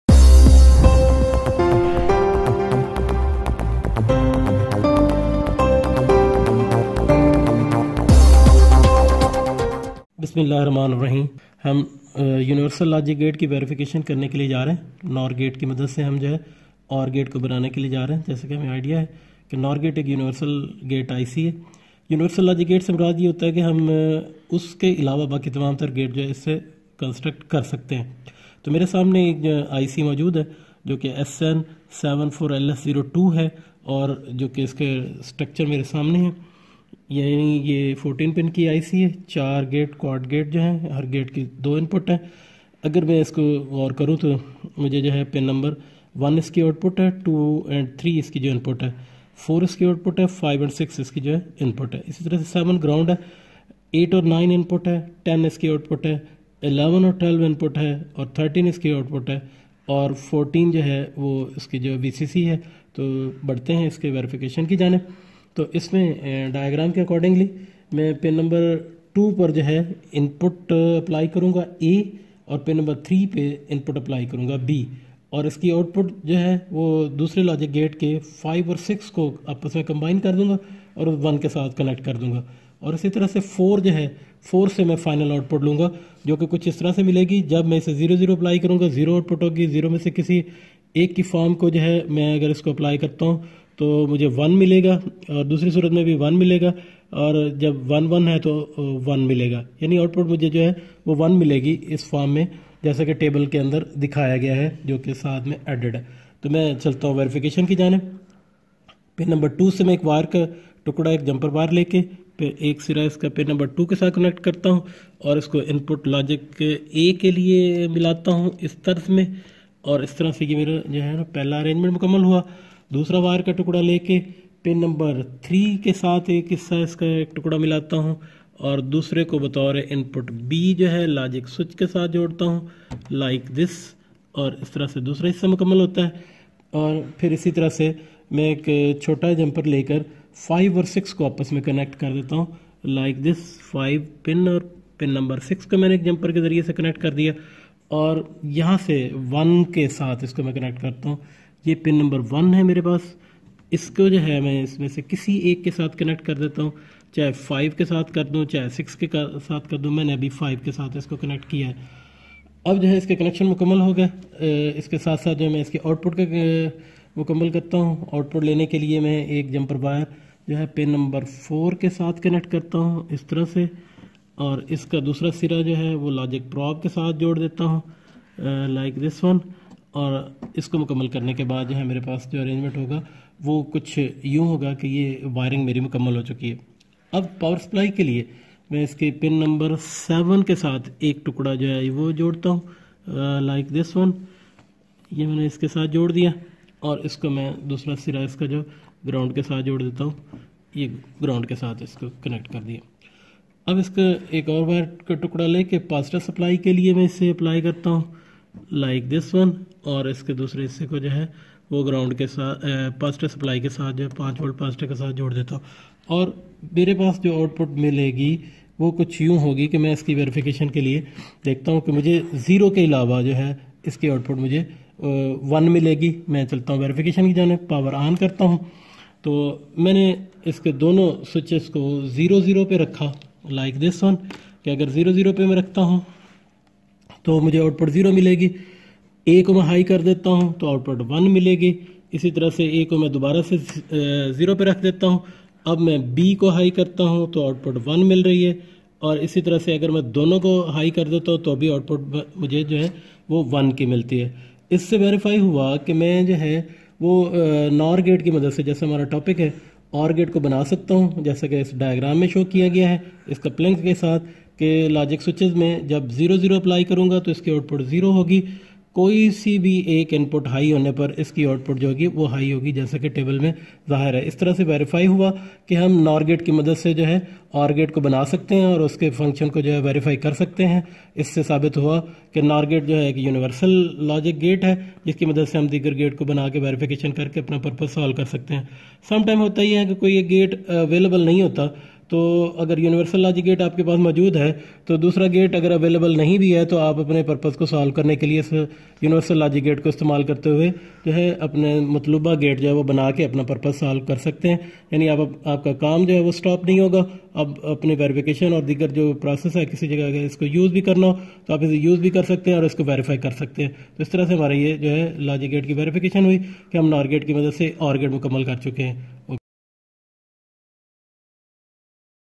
How to construct OR gate using NOR gate
Dear viewers you are watching MHB Channel01 on you tube. in this 05:23 minutes video i will show you How to construct OR gate using NOR(Universal) logic gate.It is also a part of verification of universal logic gate. For this purpose I have used IC number SH74LS02. It is 14 pins DIP series digital IC. As we already know that all logic gate can be constructed with the help of universal logic gates(NAND & NOR). I have connected whole circuit using circuit diagram.And adopted practical approach to complete this task with more information. As showed in this video IC consist of four logic gates build in it.All are having two inputs and one output.Power supply is provided on pin # 14(+ve) and pin # 7 (-ve).
Pins used for inputs are 2 & 3. And Pin used for output is 4.For this purpose only two gates are used out of four gates.